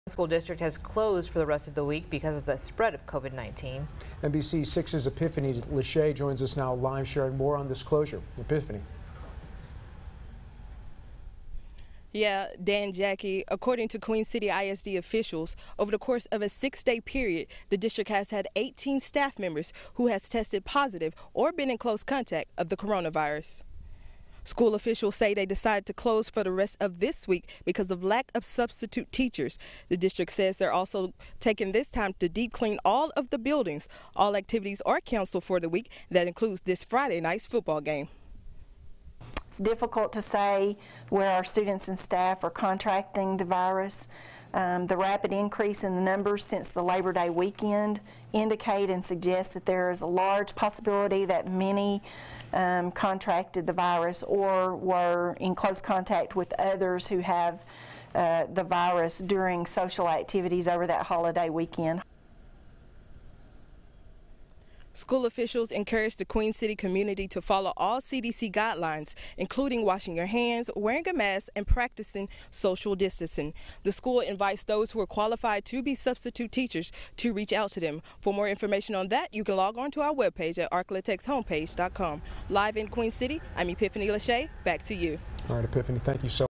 Queen City ISD shuts down all campuses due to COVID-19
The Queen City Independent School District has closed all campuses due to the coronavirus